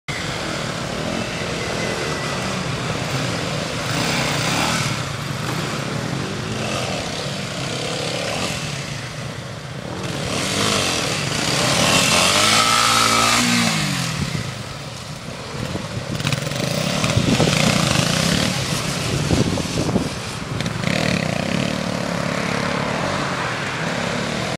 Wheely Schule beim Meister
Whelly-Days mit Lothar Schauer, Kassel 08.2013

...und für die ungeübten gibt es die "Wheely-Bar"  :-)